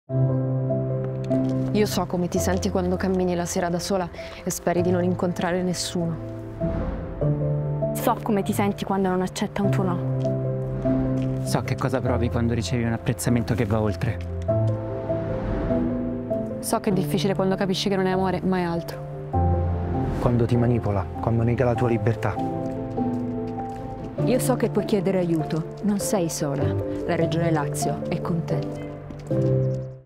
Non sei sola: la Regione Lazio al fianco delle donne
In occasione della Giornata Internazionale contro la violenza sulle donne, la Regione Lazio lancia la campagna 'Non Sei Sola', realizzata con la partecipazione dell’attrice Maria Grazia Cucinotta. Il messaggio invita tutti a non rimanere in silenzio, rimandando al sito dedicato regione.lazio.it/nonseisola